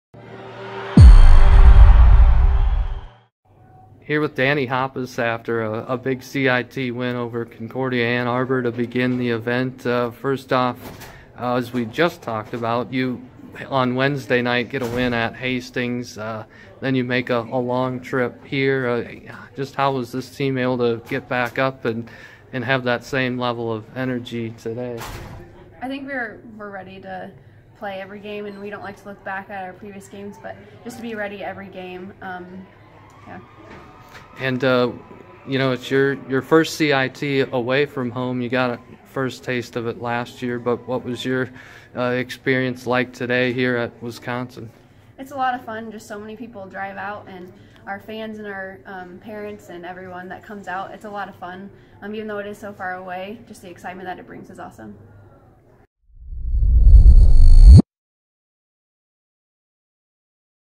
CIT POSTGAME: Dani Hoppes (Jan. 26, 2018)
Senior Dani Hoppes poured in 22 points while making six 3-point shots in a 104-66 win over Concordia-Ann Arbor at the Concordia Invitational Tournament on Jan. 26, 2018.